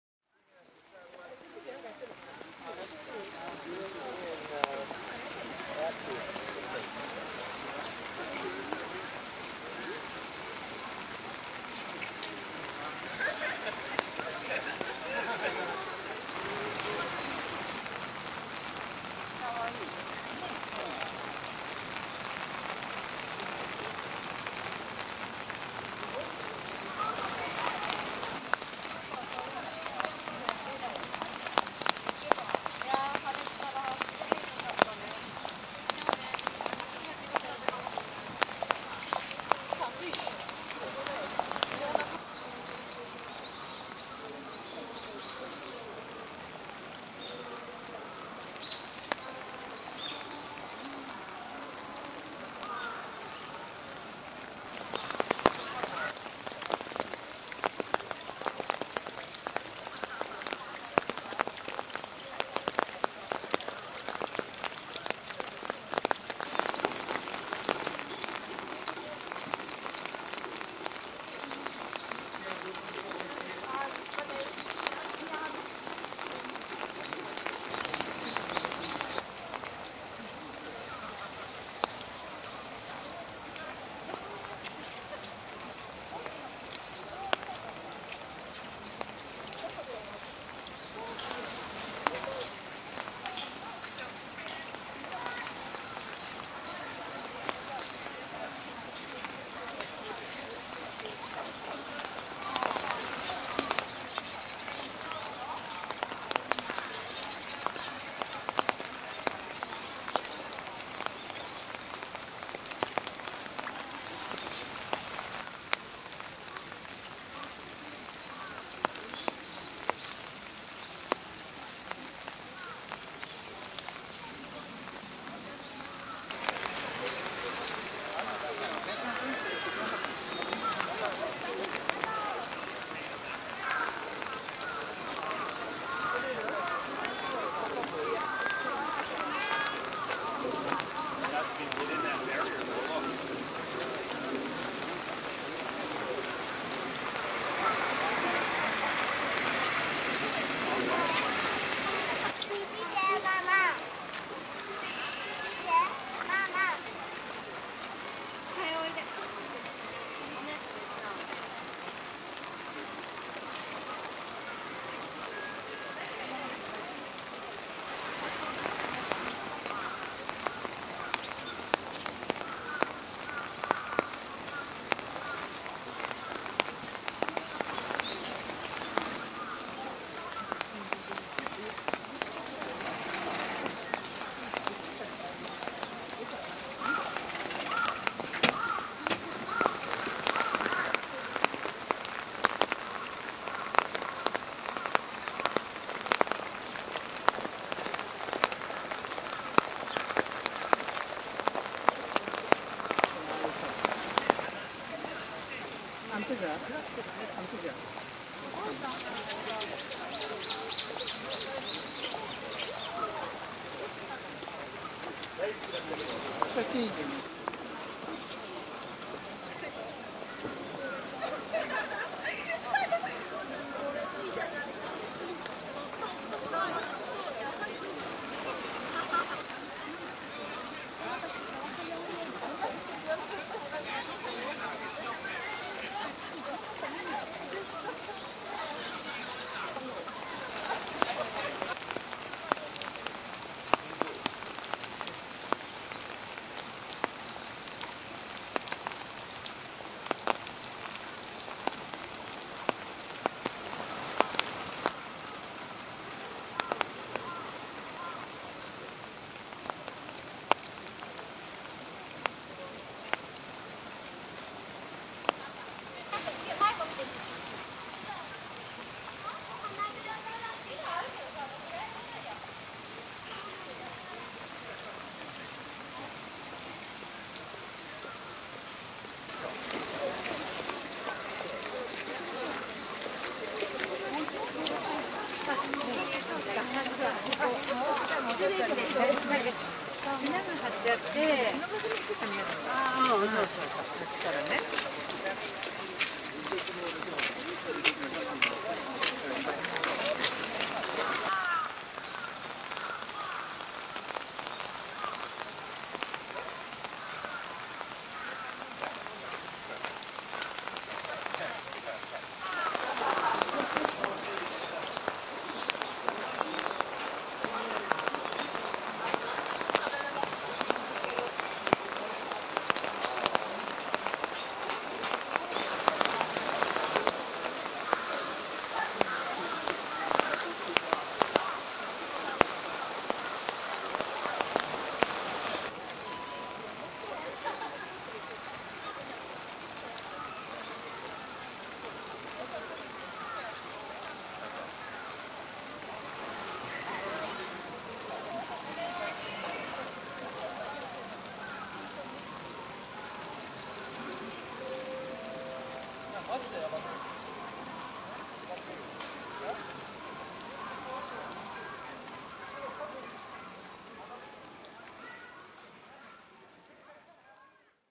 雨の上野公園でのお花見 cherry blossoms in the rain in Ueno park
2014年4月3日。雨の上野公園でお花見をしてきました。この雨で桜はちりはじめていましたが、それでも雨に濡れた桜はきれいでしたよ。撮影機材はNikon1 V1 + Nikon 1 NIKKOR (ワンニッコール) 18.5mm f/1.8 及び Nikon 望遠ズームレンズ 1 NIKKOR VR 30-110mm f/3.8-5.6 です。